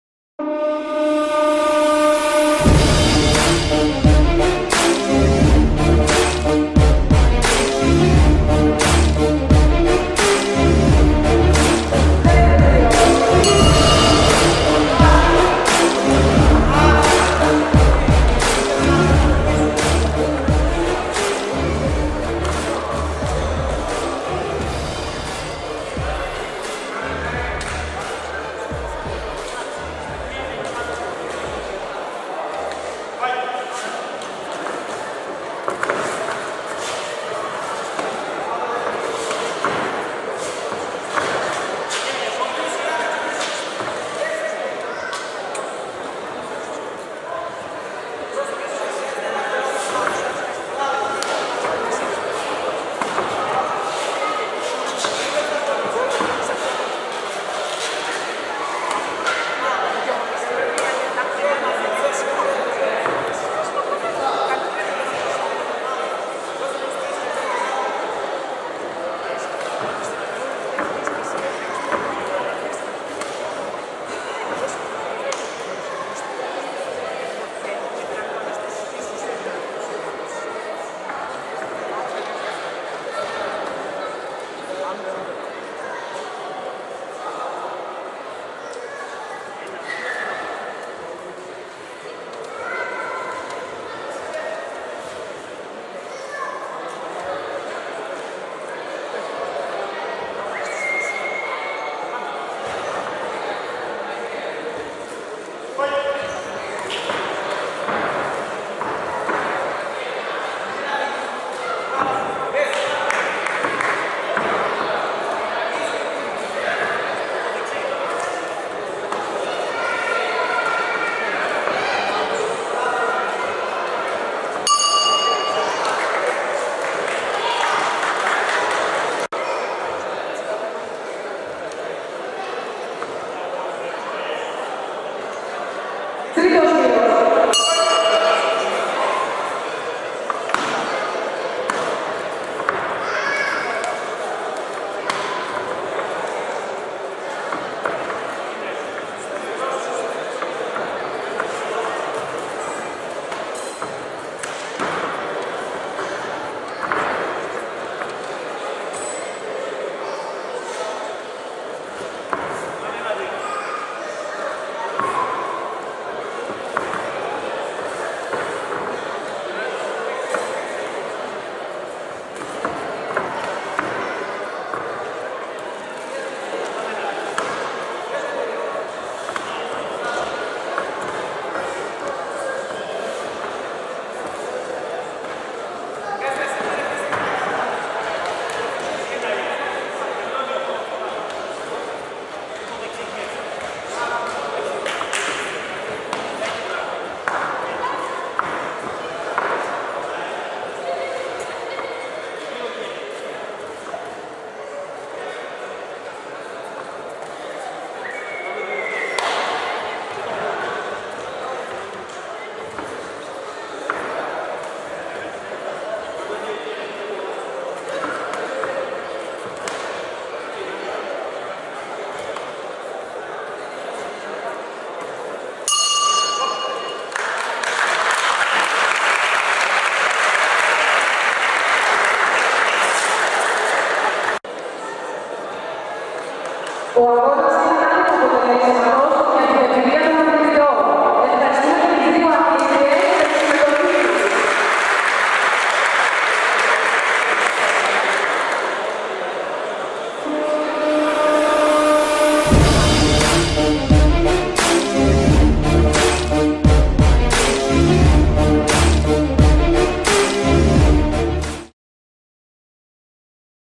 ΔΙΑΣΥΛΛΟΓΙΚΟ ΕΟΜΑ - ΚΡΗΤΗ - 14/7/2013 - ΙΩΑΝΝΑ ΤΑΒΕΡΝΑΡΑΚΗ
Αγωνες Kick Boxing EOMA.ΑΓΩΝΑΣ ΧΩΡΙΣ ΝΙΚΗΤΗ Προπονητής Κουγιουμιτζάκης Μιχάλης. Ηράκλειο Κρήτης.